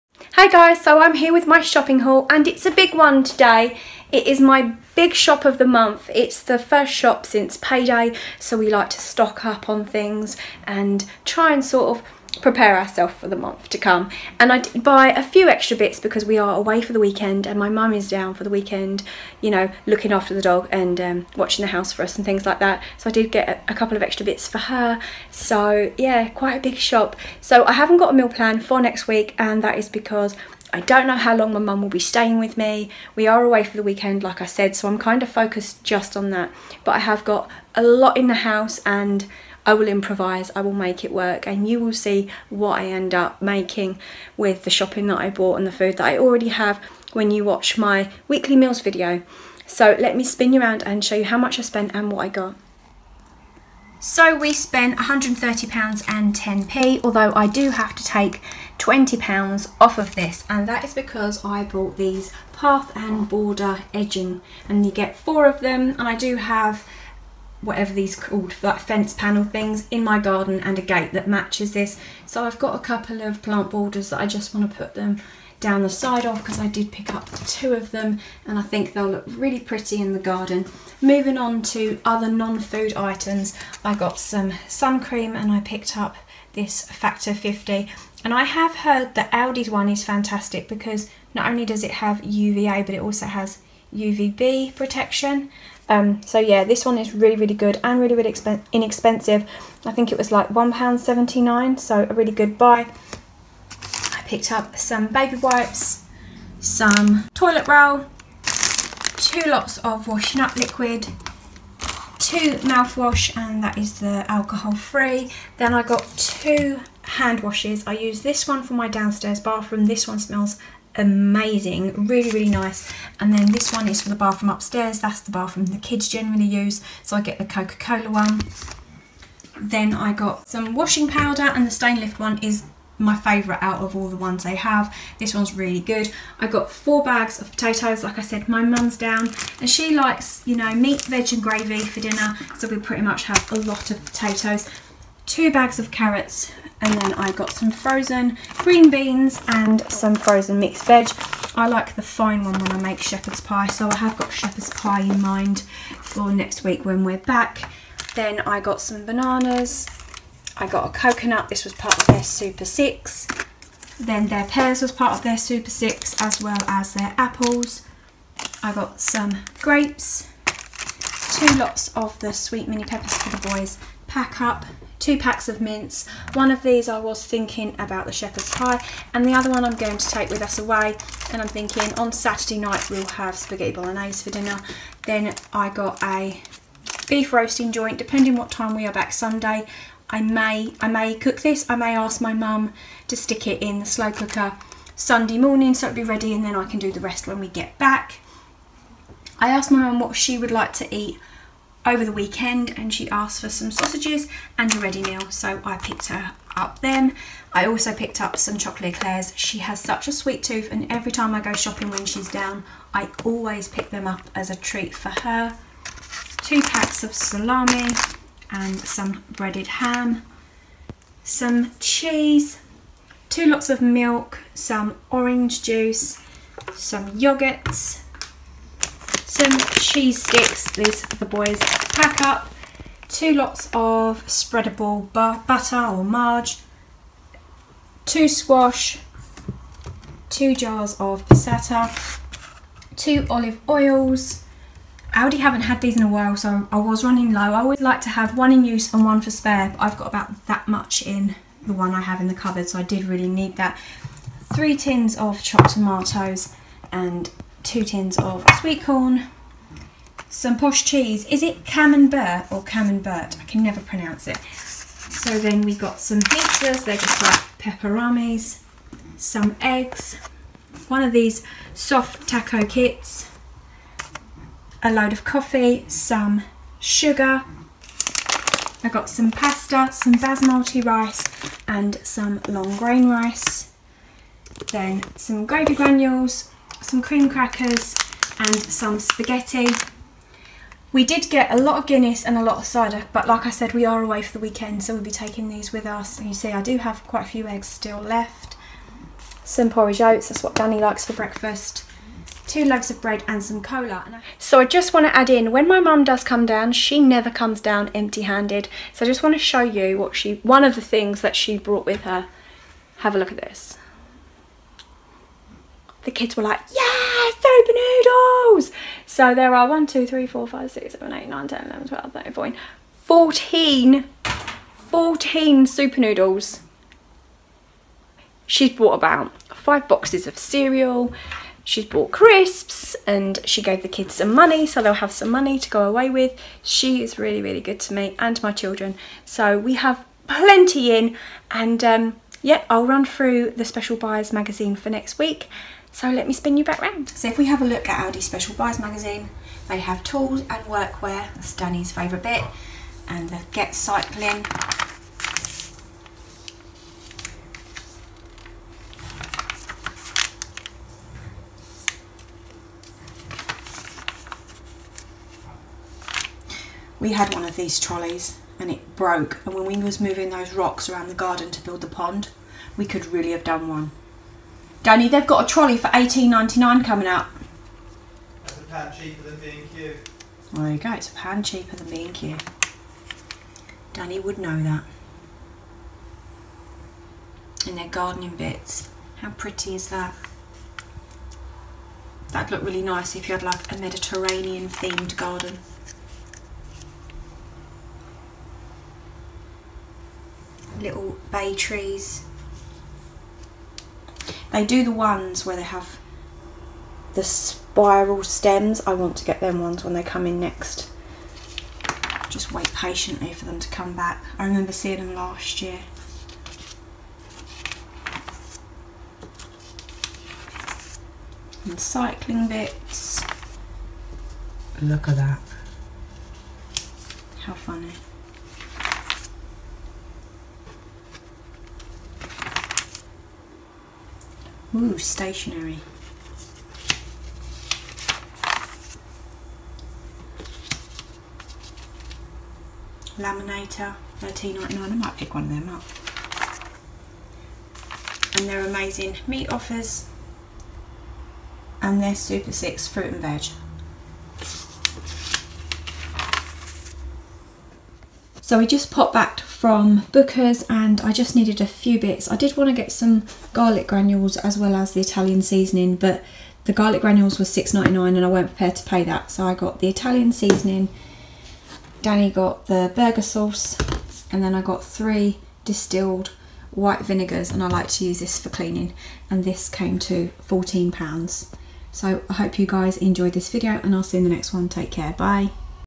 Weekly food shopping haul
My 1st food shop of the month (from my payday) I spent £130.10 in Aldi so from my monthely budget of £400 I have £269.90 remaining